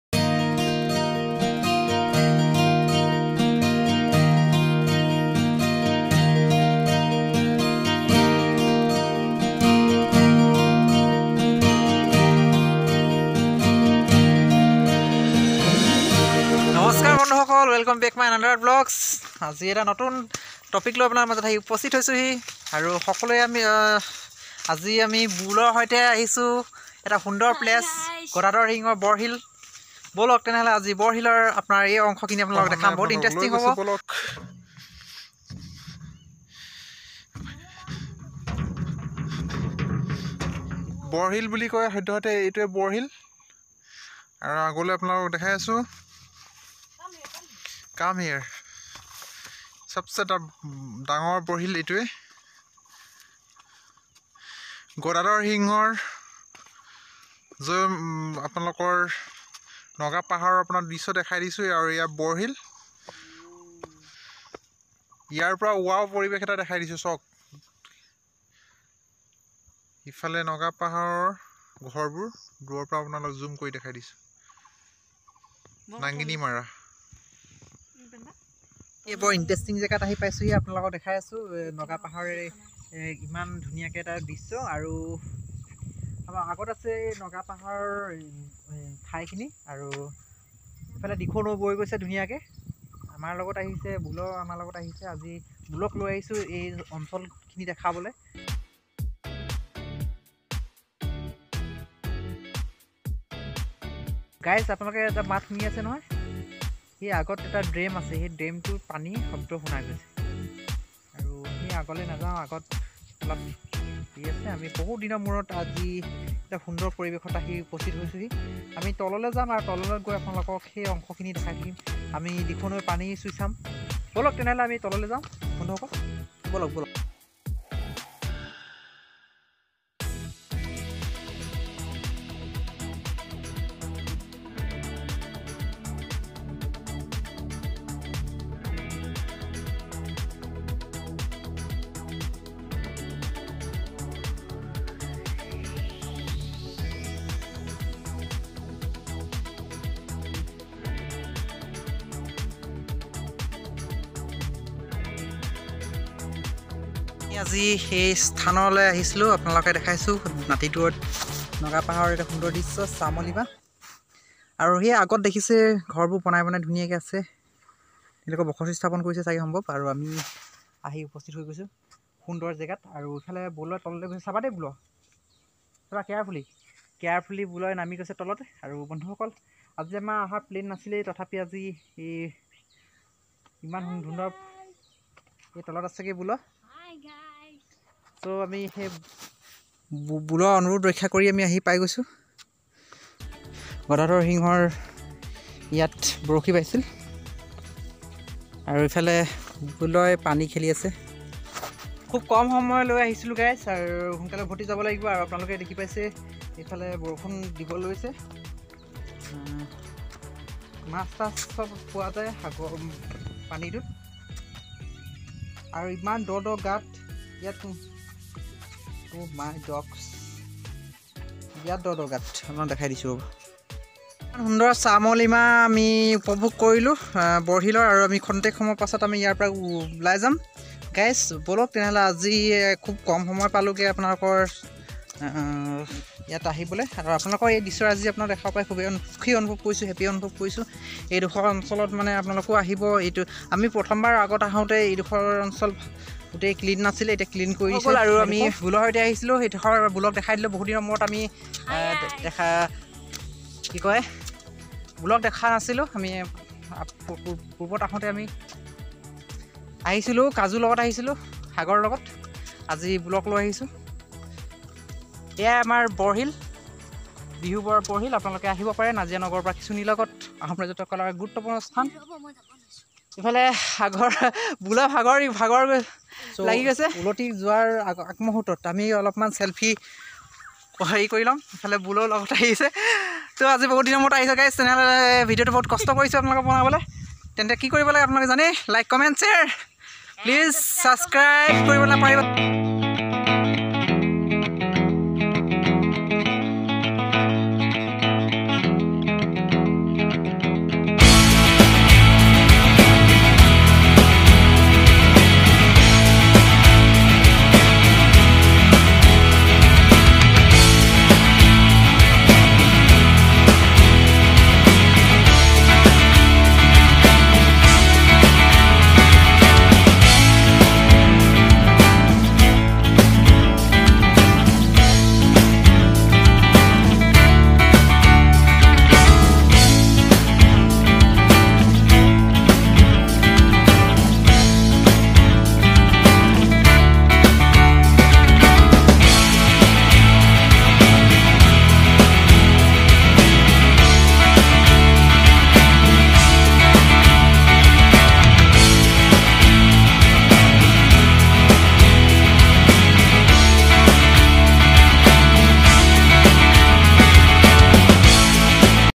Historical Place Borhill অসম-Nagaland-সীমান্ত
দালিমীৰ দেশত এদিন বুলৰ সৈতে।বৰশীৰ মাছ ধৰা দিনৰ কথাৰে ।দিখৌ নৈৰ শ্যামলীমা